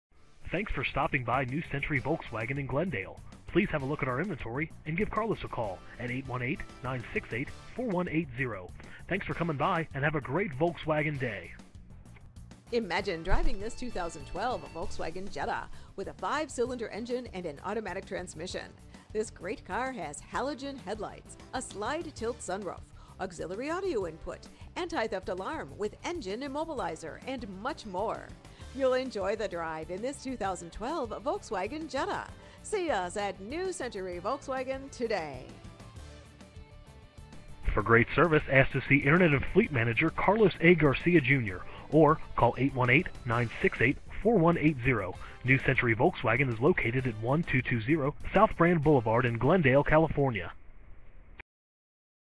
2012 Volkswagen Jetta New Century Volkswagen Glendale, CA 91204
Volkswagen Jetta Imagine driving this Black Uni Volkswagen Jetta Sedan, equipped with a 5 Cylinder engine and an automatic transmission. Enjoy this great car . Have peace of mind in this Volkswagen Jetta. See us at New Century Volkswagen today!
MILES: 19,455
VIN:3VWDP7AJ6CM307989

1220 South Brand Boulevard
Glendale, California 91204